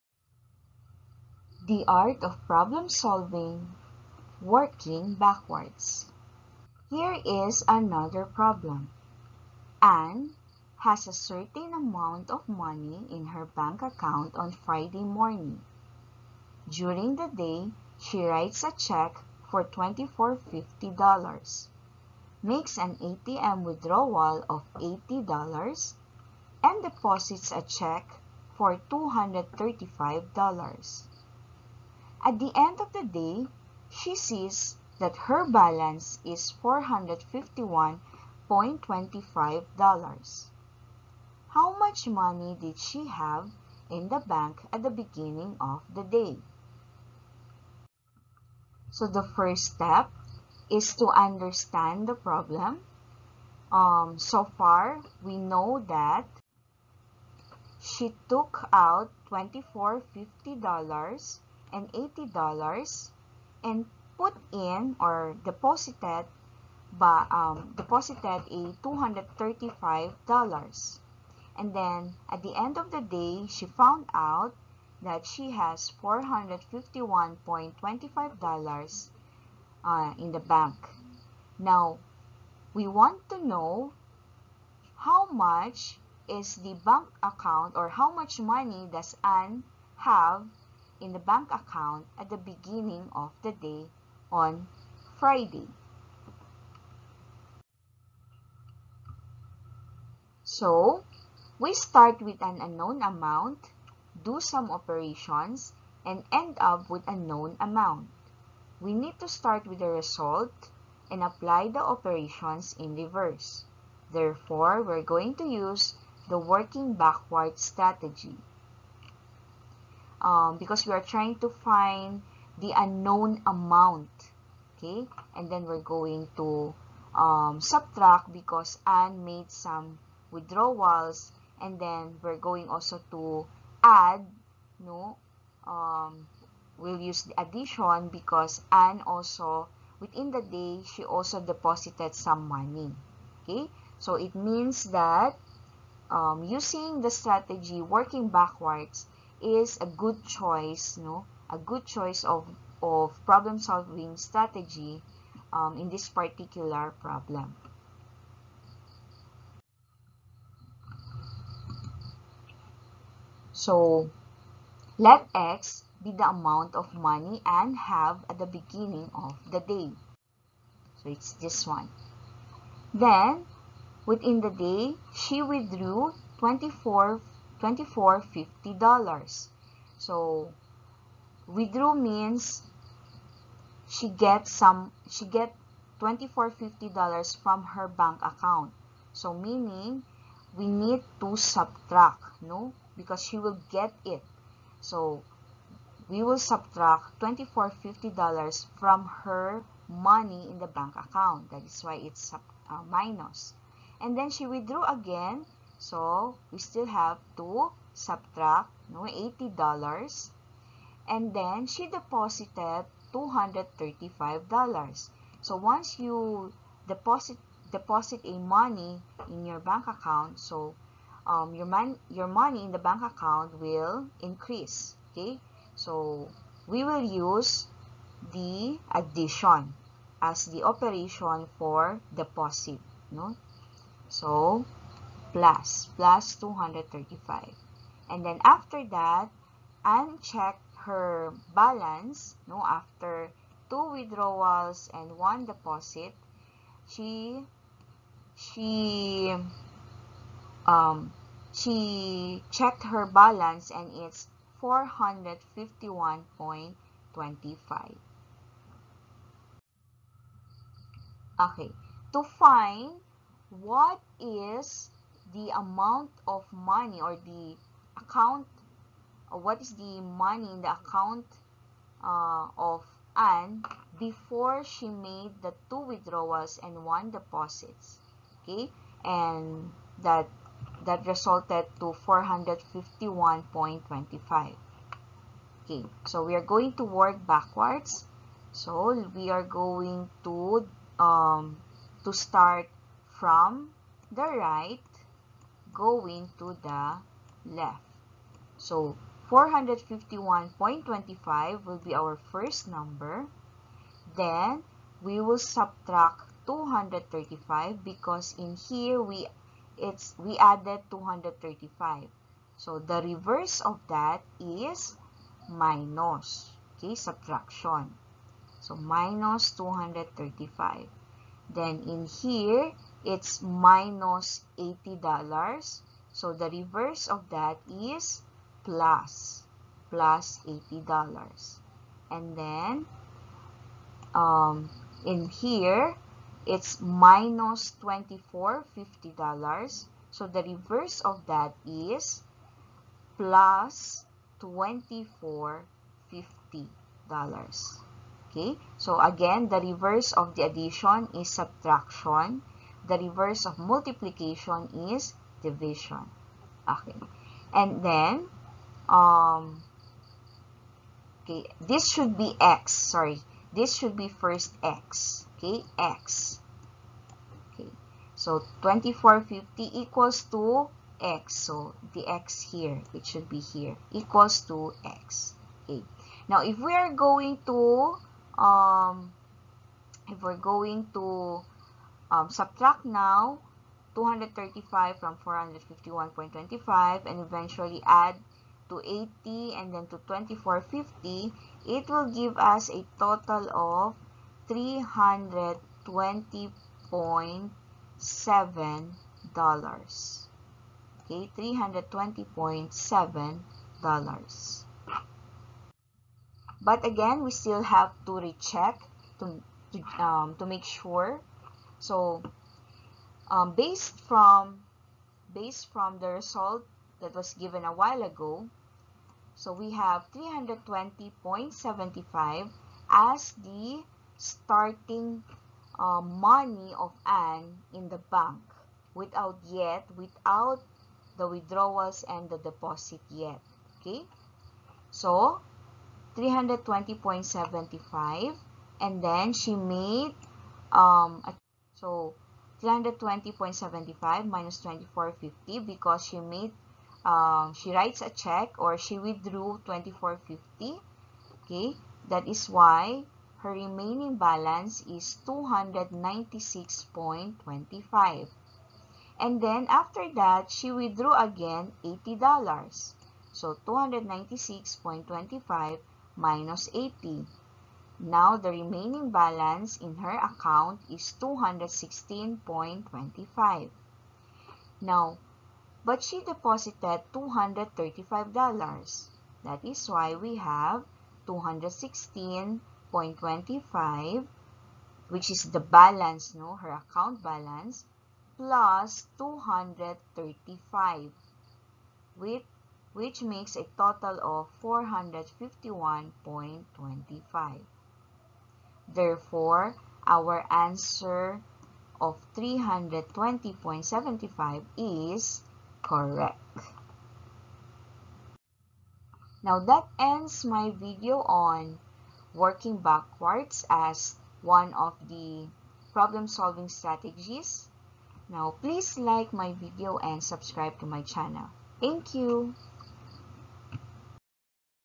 Problem Solving Strategy: Working Backwards | Mathematics in the Modern World
In this video, we explore one of the most effective problem-solving strategies in mathematics — Working Backwards! This method helps you solve complex problems by starting from the end and retracing the steps to find the solution.

This lesson, part of Mathematics in the Modern World, is perfect for college students, educators, and anyone wanting to improve their logical reasoning and problem-solving skills. You’ll learn how to apply the Working Backwards strategy to real-life mathematical problems and see why it’s a powerful tool in analytical thinking.

🎯 What You’ll Learn:
✅ What is the “Working Backwards” strategy in problem solving
✅ Step-by-step guide on how to use this method
✅ Sample problems and detailed solutions
✅ Real-life applications of the working backwards strategy
✅ How it improves logical and critical thinking

💡 Watch until the end for sample problems, explanations, and a quick quiz to test your understanding of this strategy!